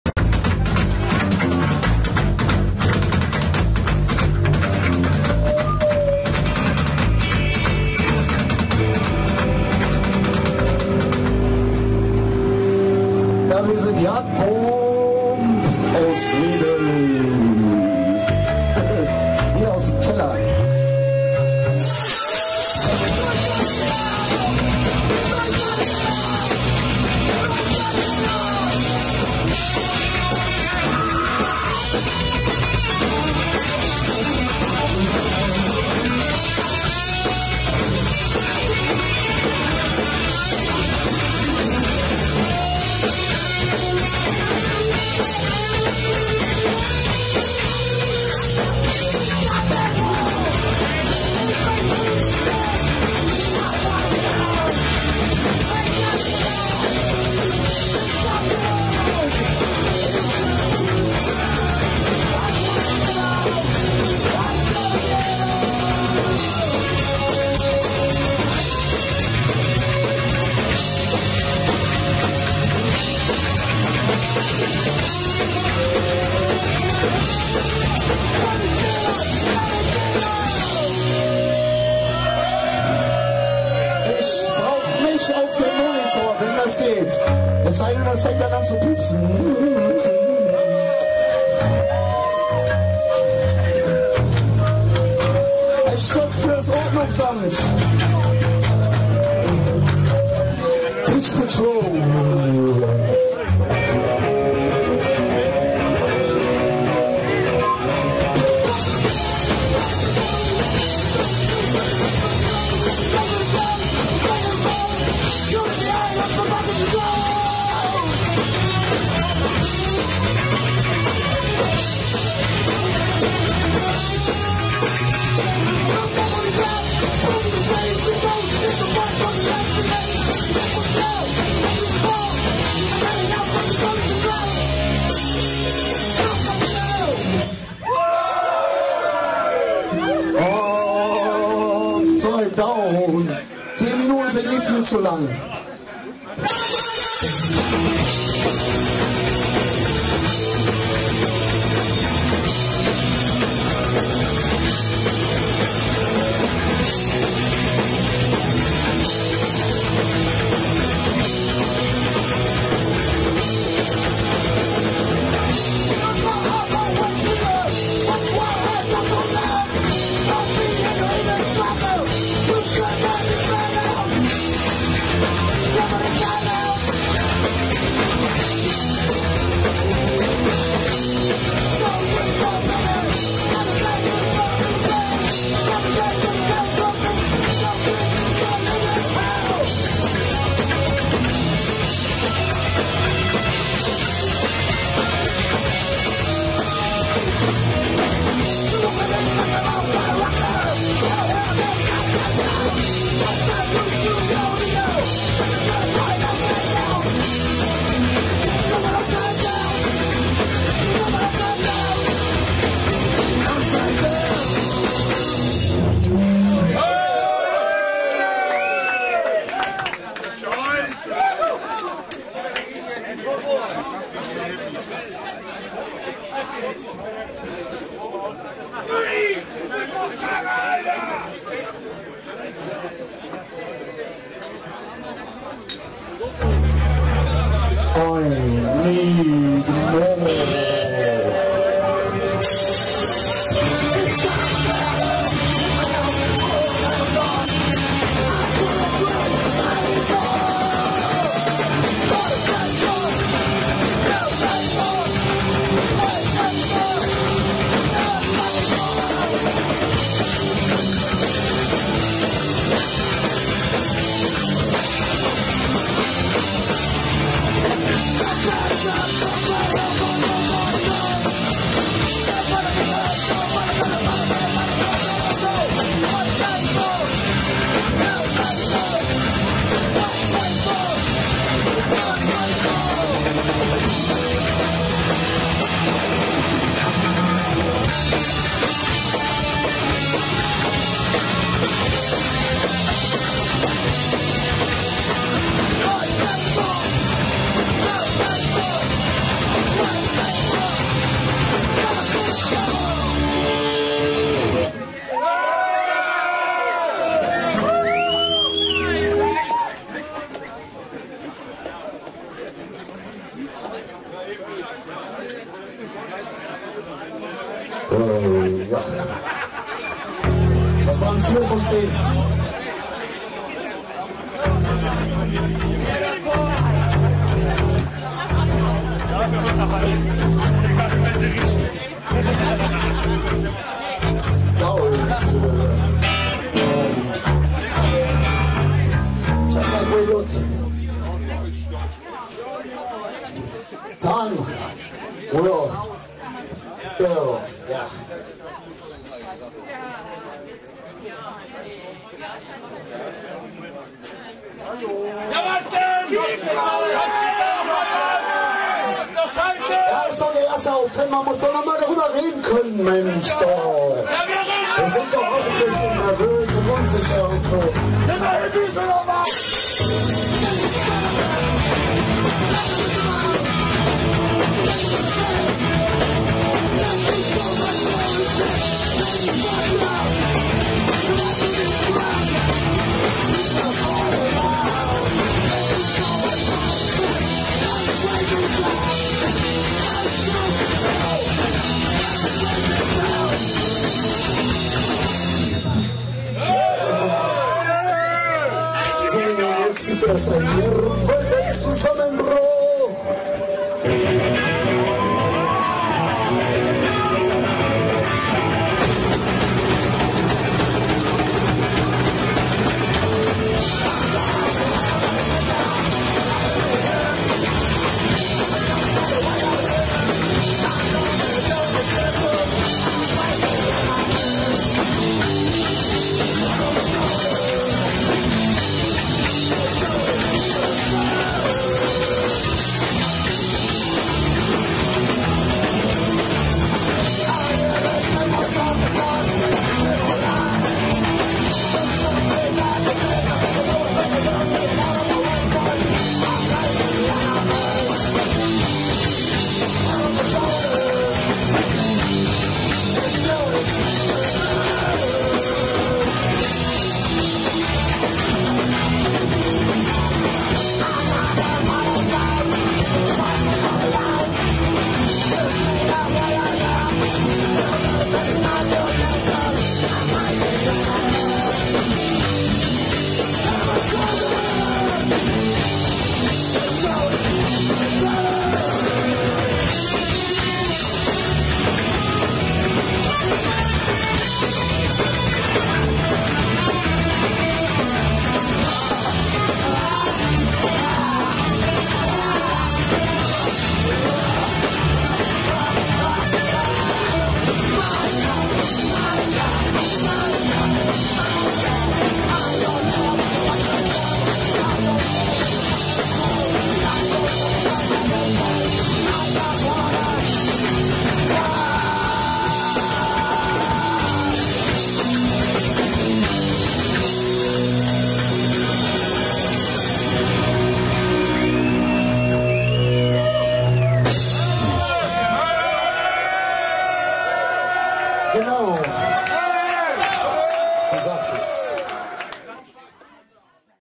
YArd Bomb Villa Wedel 17.11.2012 Part 1
Erster Auftritt von der Band YARD BOMB in der Villa Wedel. Bombe Auftritt, sag´ ich ma´!

THX TO BENNY!

-SCHEISZE FANZINE